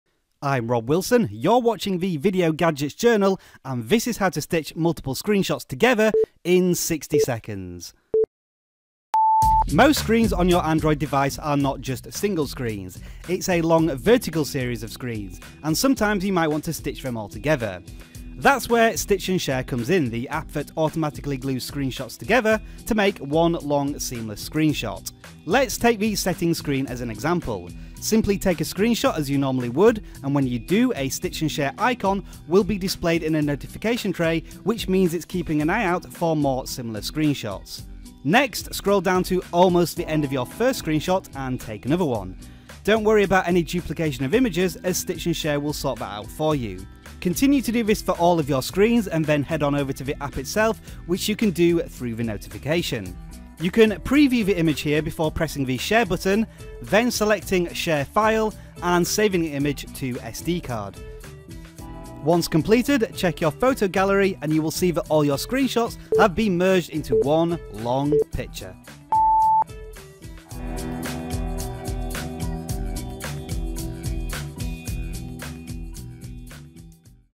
How to Stitch Multiple Android Screenshots into One in 60 Seconds
Have you ever wanted to take multiple screenshots on your Android device and automatically stitch them together? Well now you can in just 60 seconds!